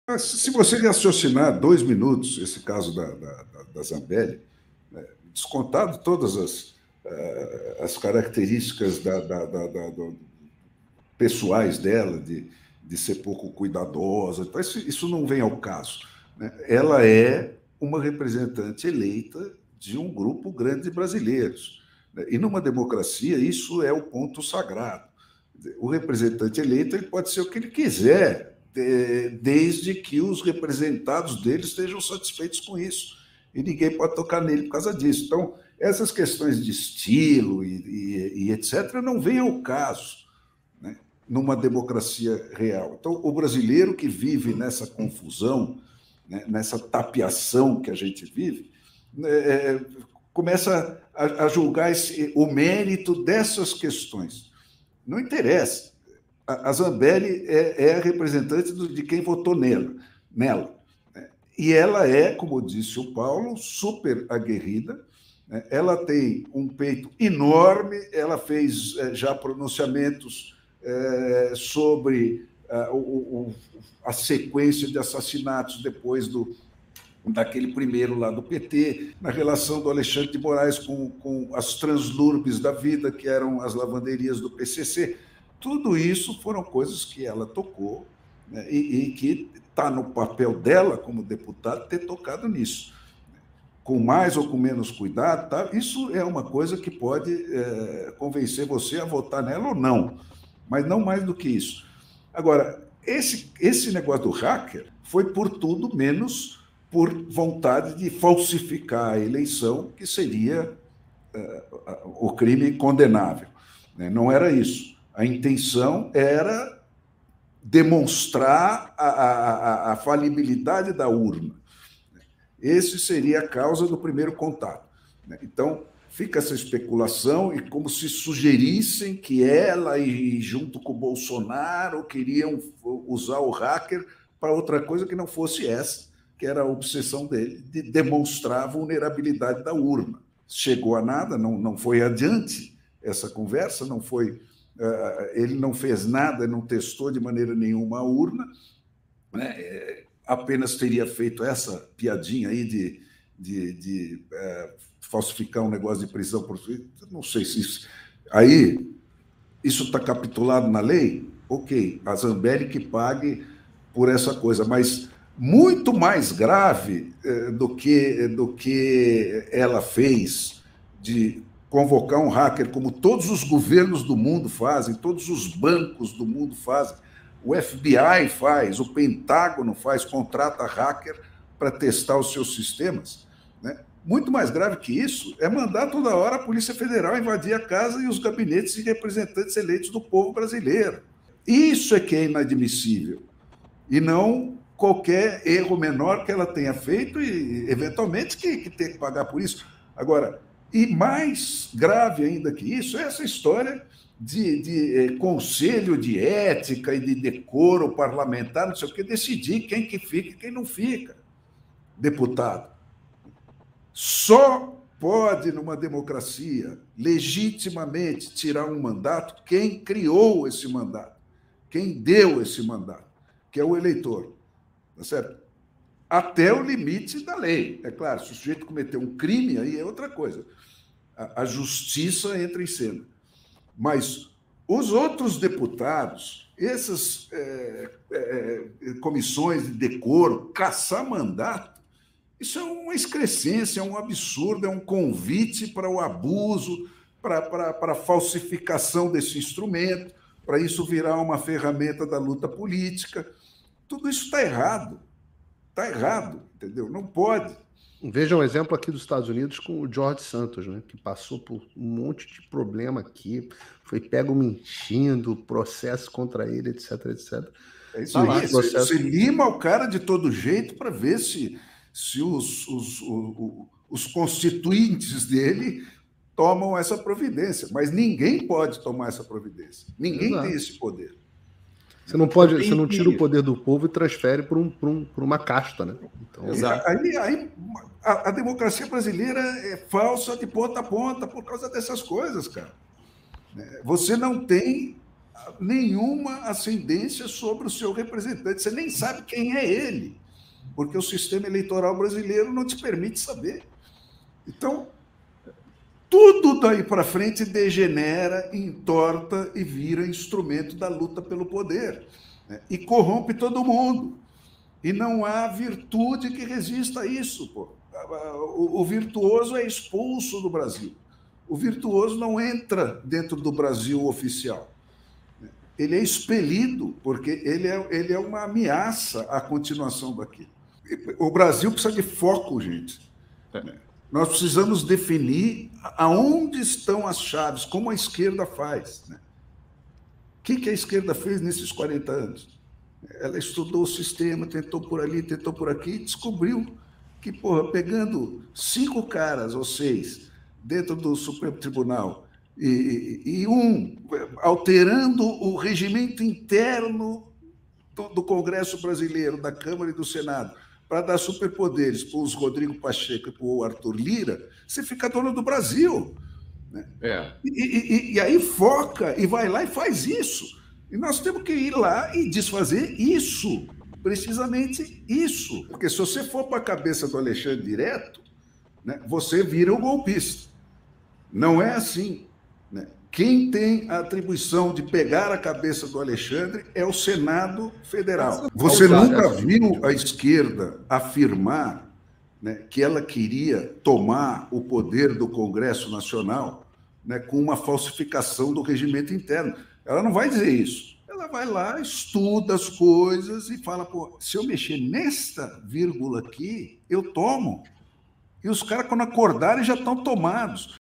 O CASO ZAMBELLI E NOSSA DEMOCRACIA FAJUTA
Carla Zambelli só deve a piadinha que fez com AdeM. Tudo o mais que aconteceu com ela só prova quanto é falsa a nossa "democracia"